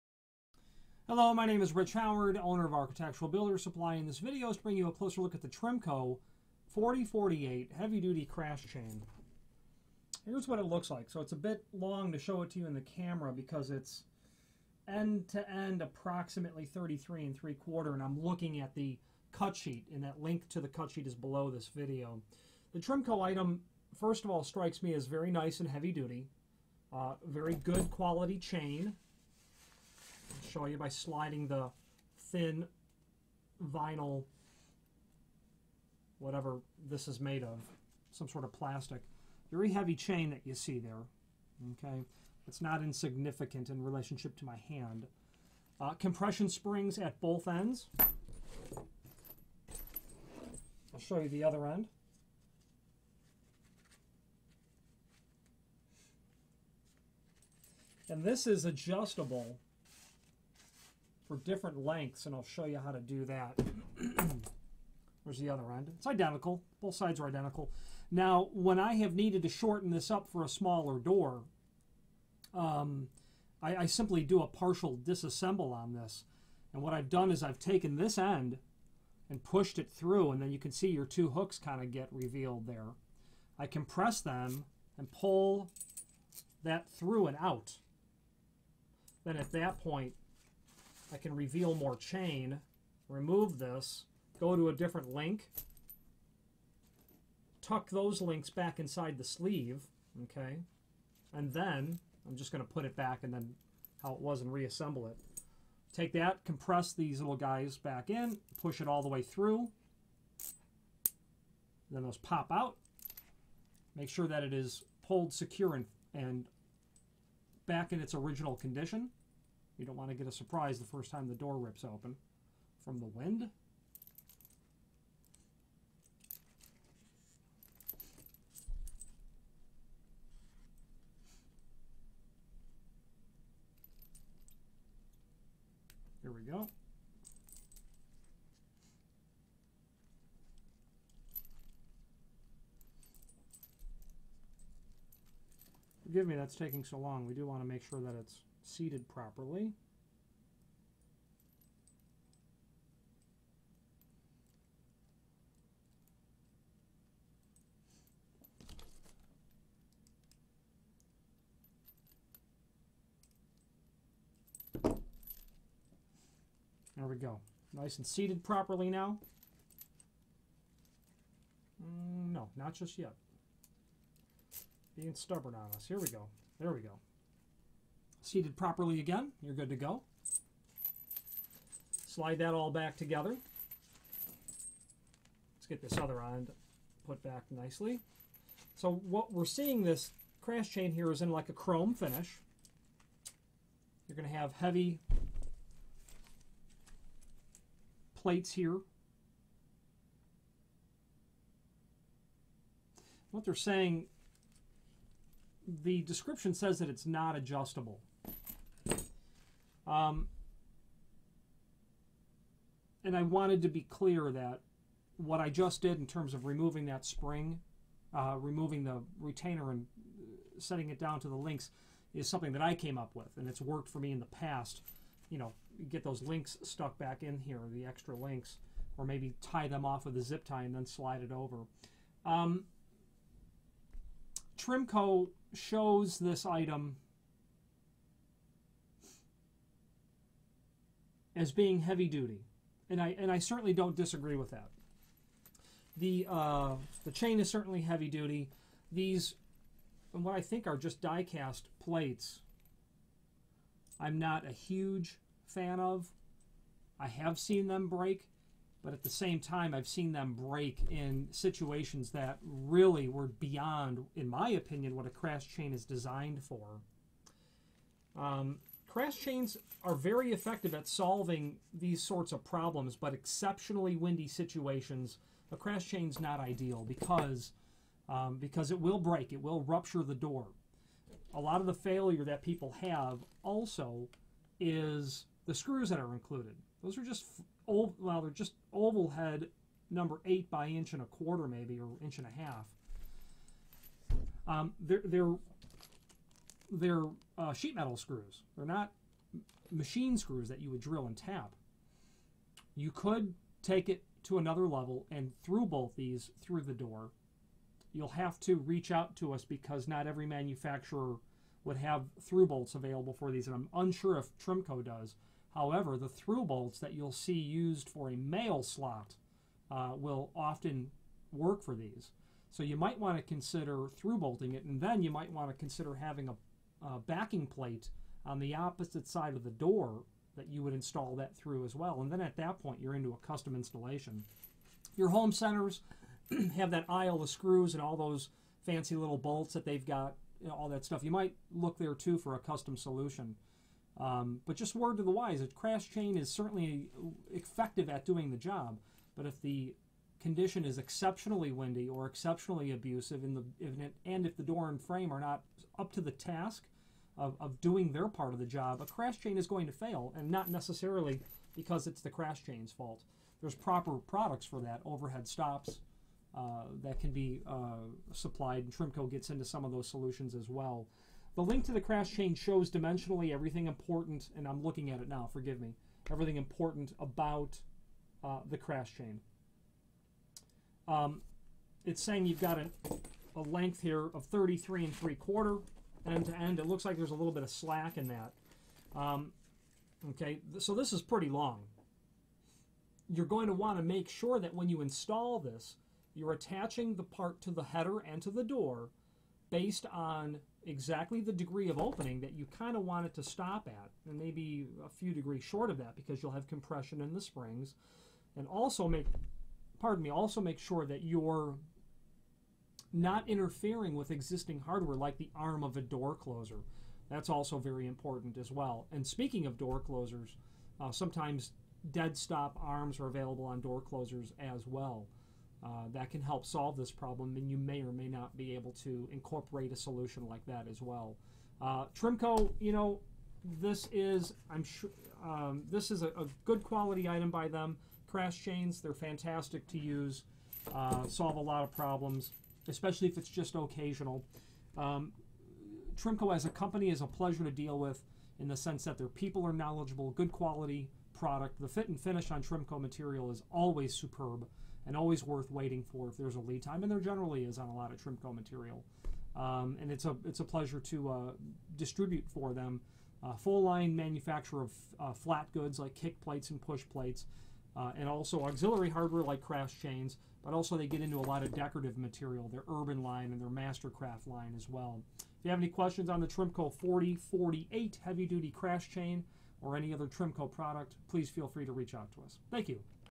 Trimco 4048 Heavy Duty Crash Chain
This video is to bring you a closer look at Trimco 4048 Heavy Duty Crash Chain.  Here's what it looks like.  It's a bit long to show it to you in the camera because it's end to end approximately 33-3/4" and I'm looking at the cut sheet.  A link to the cut sheet is below this video.  The Trimco item, first of all strikes me as very nice and heavy duty.  A very good quality chain, I'll show you by sliding the thin vinyl, whatever this is made of some sort of plastic.  Very heavy chain that you see there.  It's not insignificant in relationship to my hand.  Compression springs at both ends.  I'll show you the other end.  This is adjustable for different lengths and I'll show you how to do that.  Here's the other end.  Identical, both sides are identical.  Now when I have needed to shorten this up for a smaller door, I simply do a partial disassembly on this.  And what I've done is I've taken this end and pushed it through.  You can see the two hooks get revealed there.  I can press then and pull that through and out.  At that point, I can reveal more chain, remove this and go into a different link.  Tuck those links back inside the sleeve.  And then, I'm just going to put it back how it was and reassemble it.  Take that and compress these little guys back in, push it all the way through, then those pop out.  Make sure that it is pulled secure and back in its original condition.  You don't want to get a surprise the first time the door rips open from the wind.  There we go.  Forgive me that it's taking so long. We do want to make sure that it's seeded properly.  There we go.  Nice and seeded properly now. No, not just yet.  There we go, there we go.  Seeded properly again, and you're good to go.  Slide that all back together.  Let's get this other end put back nicely.  So what we are seeing is this crash chain is in a chrome finish.  You're going to have heavy plates here.  What they are saying is. The description says they are not adjustable and I wanted to be clear on that.  What I just did in terms of removing that spring, removing the retainer and setting it down into the links is something that I came up with and it has worked for me in the past.  Get those links stuck back in here, the extra links or maybe tie them off with a zip tie and slide it over.  Trimco shows this item as being heavy duty.  I certainly don't disagree with that.  The chain is certainly heavy duty.  These are what I think are die cast plates, I'm not a huge fan of.  I have seen them break, but at the same time I've seen them break in situations that really went beyond in my opinion what a crash chain is designed for.  Crash chains are very effective at solving these sorts of problems, but exceptionally windy situations a crash chain is not ideal because it will break and rupture the door.  A lot of the failure that people have also is the screws that are included.  Those are just oval head #8 by 1-1/4" maybe or 1-1/2".  They're sheet metal screws, they are not machine screws that you would drill and tap.  You could take it to another level and screws bolt these through the door.  Unsure if Trimco does, however the through bolts that you will see used for mail slots will often work for these.  So you might want to consider through bolting it and then you might want to consider a backing plate on the opposite side of the door that you would install that through as well.  At that point you are into a custom installation.  Your home centers have that aisle of screws and all those fancy little bolts that they've got and all that stuff.  .  Thank you.